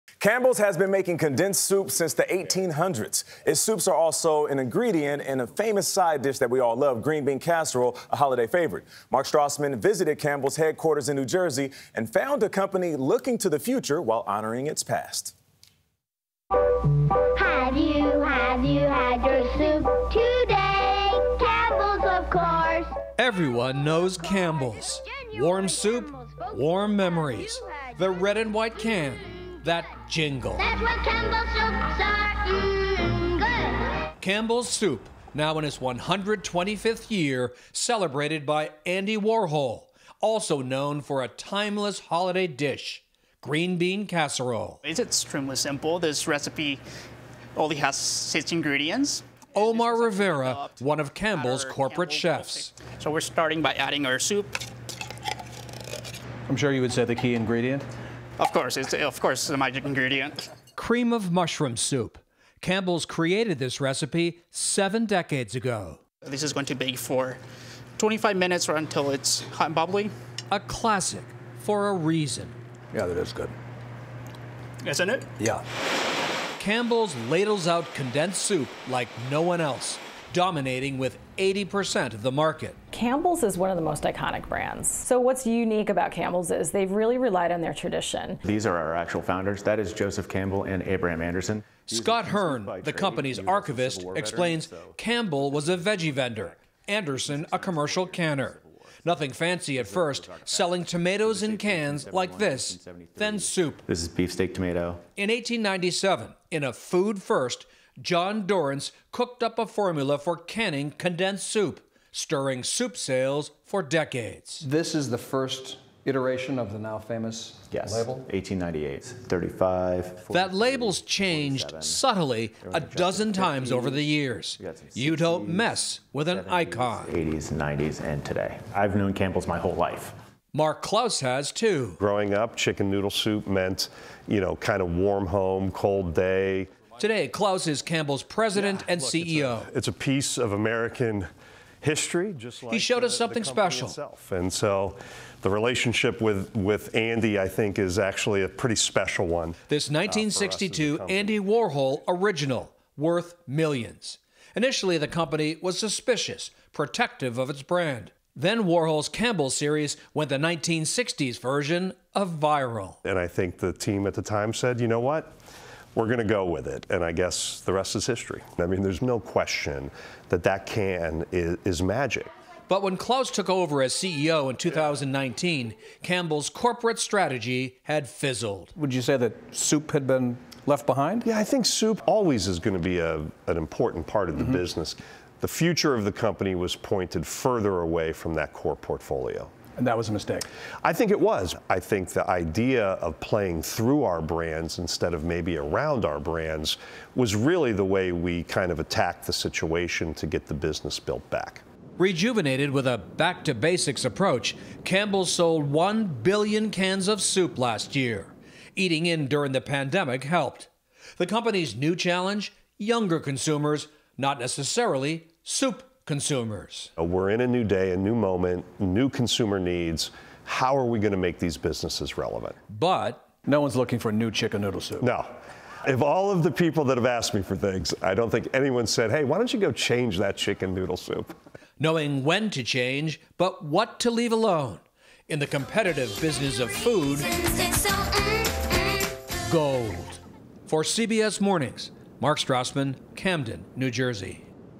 A look at the history of Campbell's soup
Campbell's Soup has been canning for more than 150 years. Mark Strassman visits Campbell's headquarters to find a company rooted in the past but looking to the future.